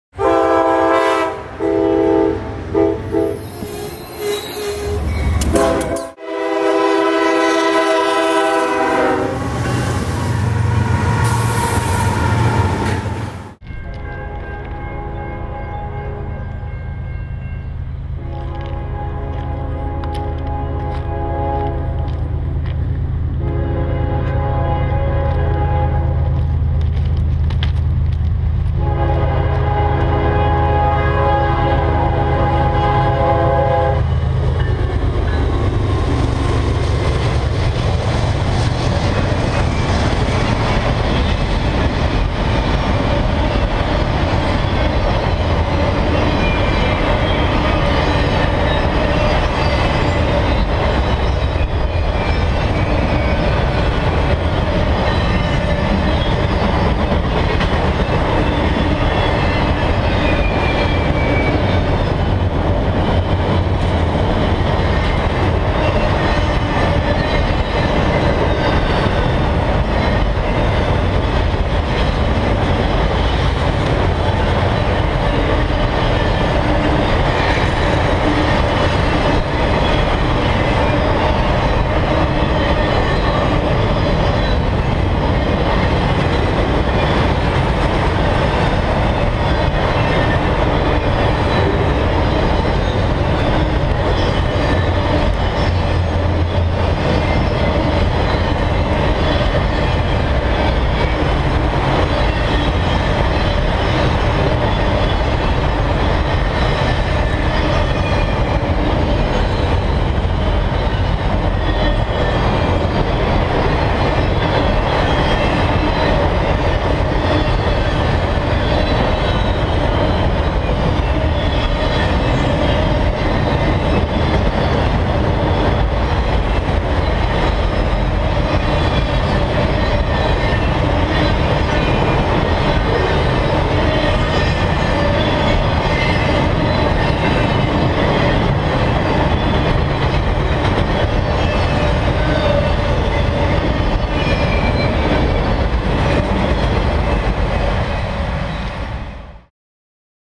NS 7604 leads train (NS 15J)
Out with Brett and Tyler for a week in PA.  Train 98 of the trip is NS 15J.